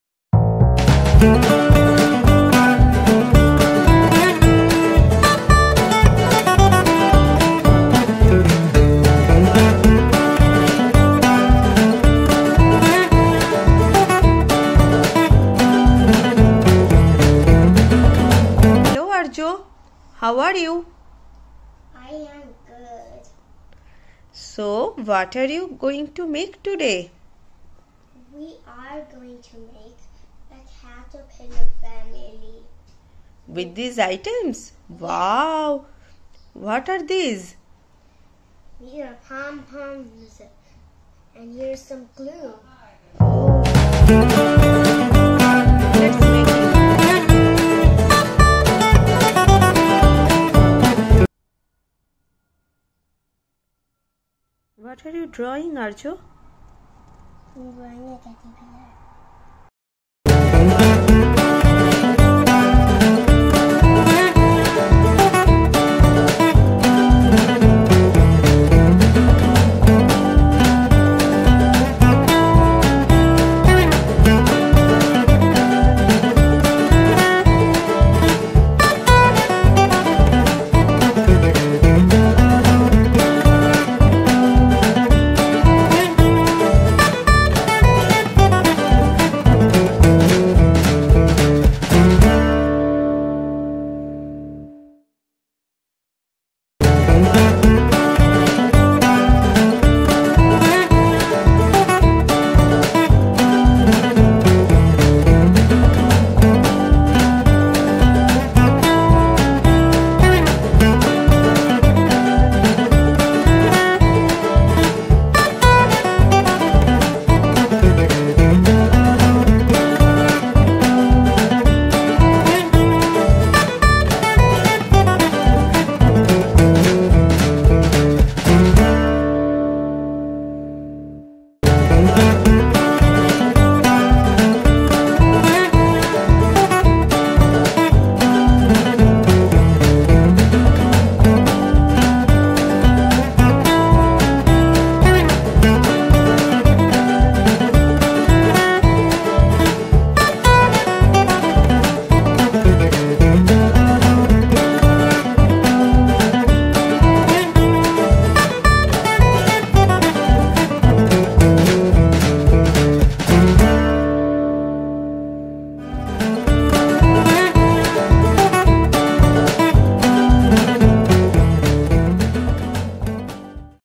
Easy Caterpillar Making For Kids
Today Arjo is making a colourful caterpillar family with pompom balls.

This type of engaging activity helps to develop child's imagination power as well as Hand-Eye coordination and Fine Motor Skill.

To get more ideas please join us at Facebook